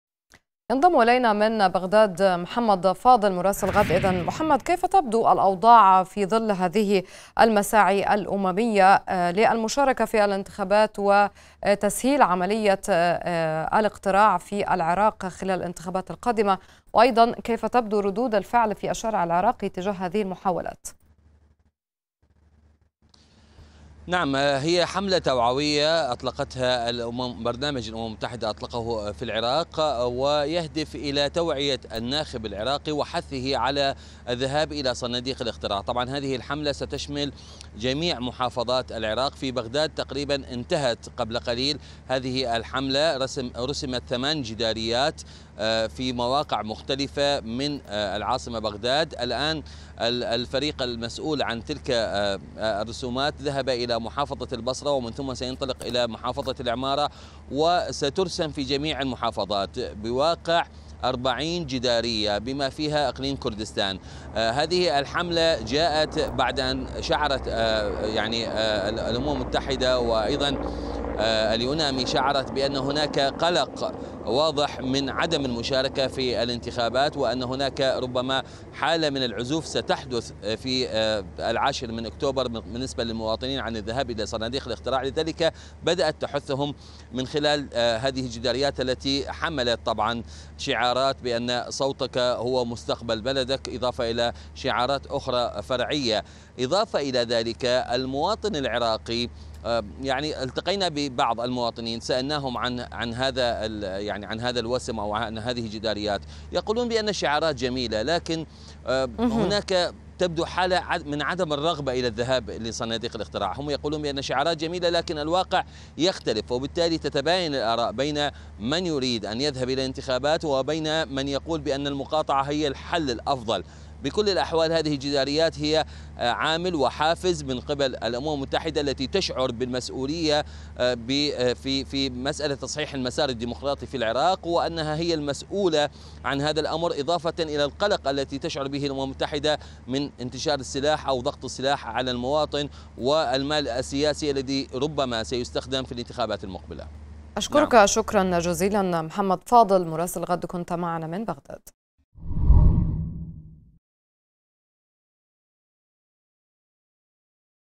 البعثة الأممية تطلق حملة للتشجيع على التصويت في الانتخابات العراقية
أطلقت بعثة الأمم_المتحدة في العراق "يونامي"، حملة واسعة لتشجيع الناخبين العراقيين على المشاركة، والتصويت في الانتخابات العامة المبكرة المقررة في العاشر من أكتوبر المقبل.
ونشرت البعثة جداريات ضخمة في العاصمة العراقية بغداد عليها عبارات توعية وشعارات محفزة للناخب العراقي، حول ضرورة مساهمته في الانتخابات، وممارسة حقه في تقرير مصير بلده.
المزيد من التفاصيل يتحدث مراسل الغد من بغداد، محمد فاضل، عبر النشرة الإخبارية.